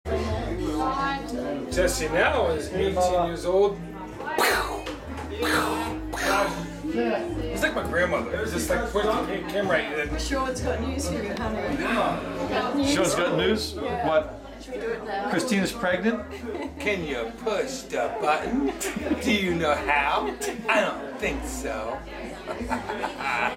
Sean and Debra
Sean Strikes Out...Again...at Jay Miller's 65th Surprise Birthday Party